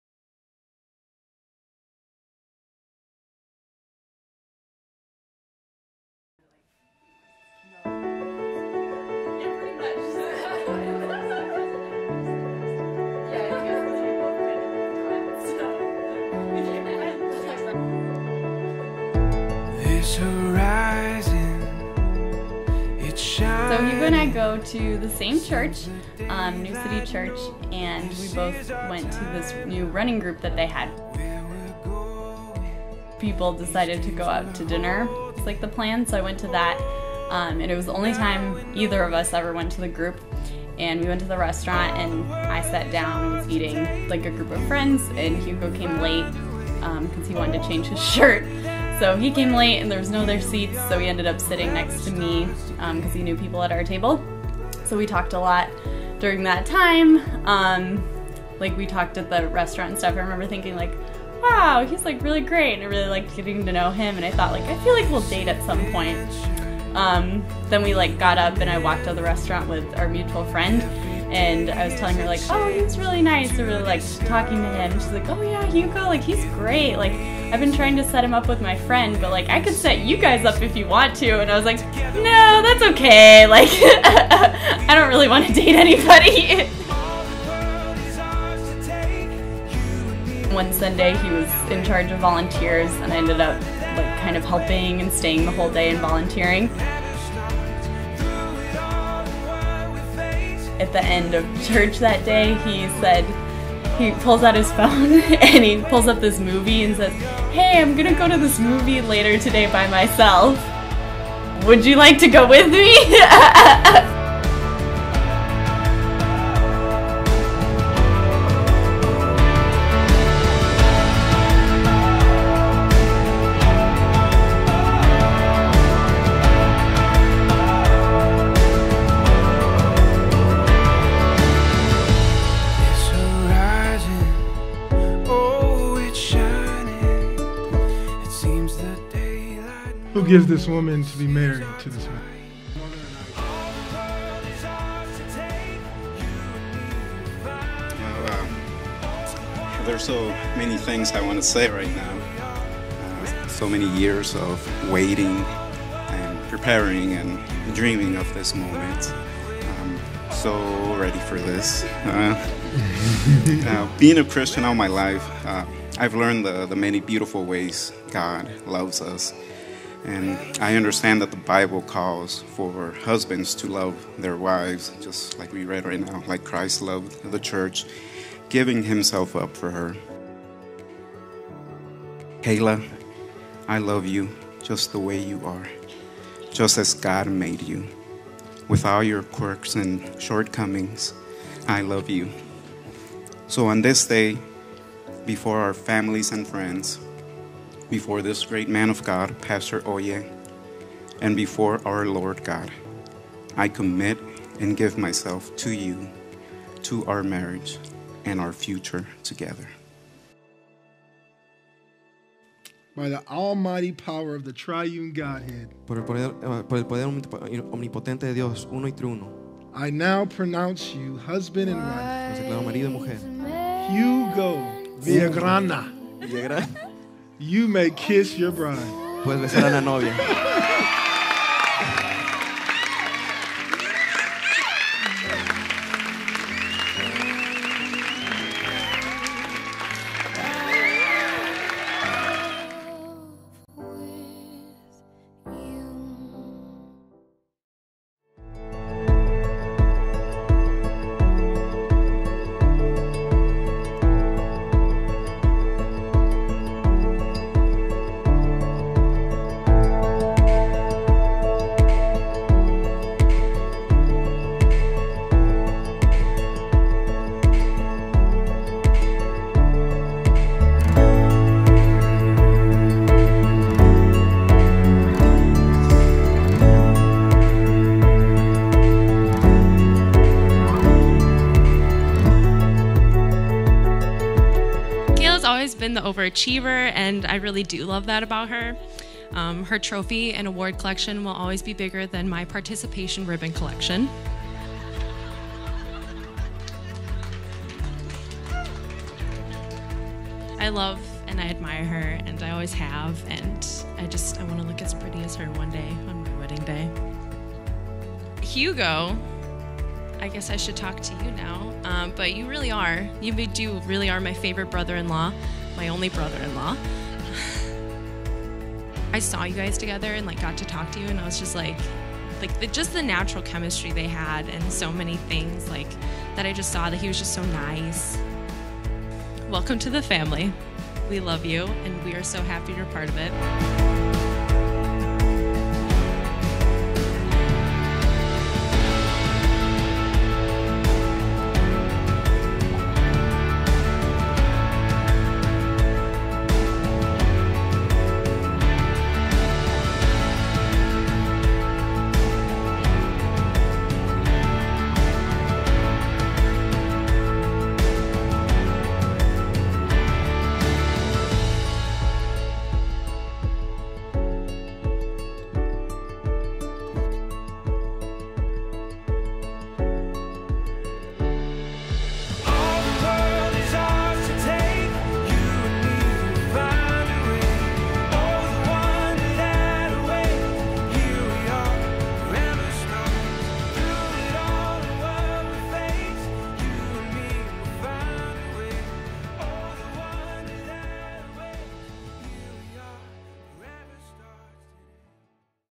Faith, The Wedding Film of Kalah & Hugo Villagrana
A wedding film shot for our great friends, Kalah, and Hugo in New City Church, Phoenix, Arizona on December 17, 2016.